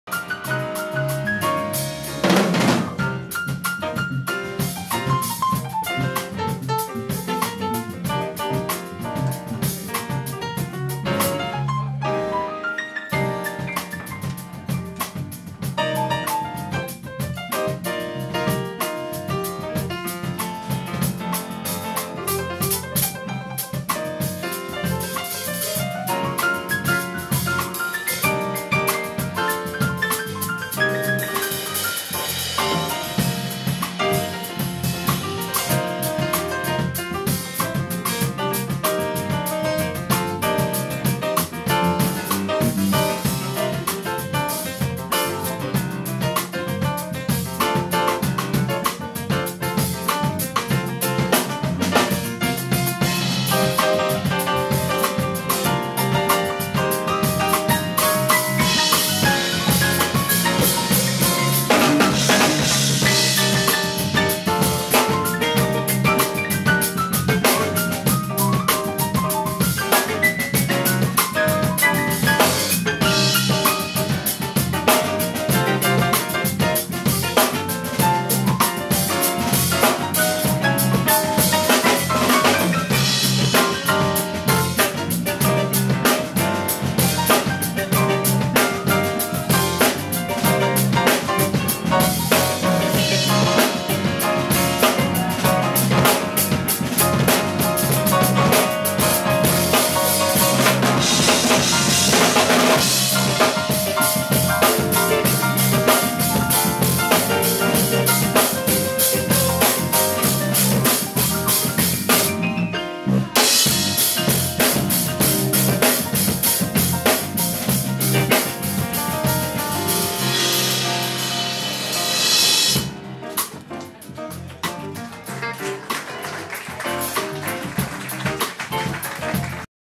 Lola's Day Off - 606 Club - Carl Hudson on Keys
Carl Hudson - great performance with Lola's Day Off at the 606 Club. Enjoy!